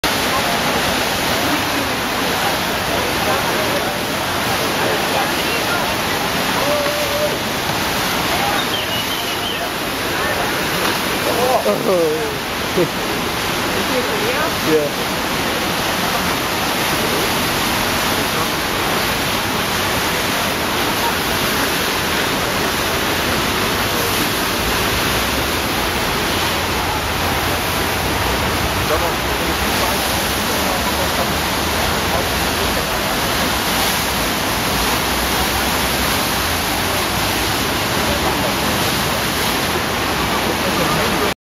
Surfing the Eisbach in Munich
I took this video while in Munich recently.

Germany has coastline along both the North and Baltic Seas, and yet its best surfing is found in southern, landlocked Munich.

The Eisbach is a small canal that flows through the city's famous English Garden.  The canal surges over a bulge in the stream bed, creating a standing wave of a good three feet high.

Surfers from all around jump onto their boards from the concrete bank of the canal and the river's flow rate provides enough boost to let the surfboards glide on the surface.

For years the sport has been prohibited on the river, but recently, state and city officials have lifted the ban.This has become a major tourist attraction in Munich.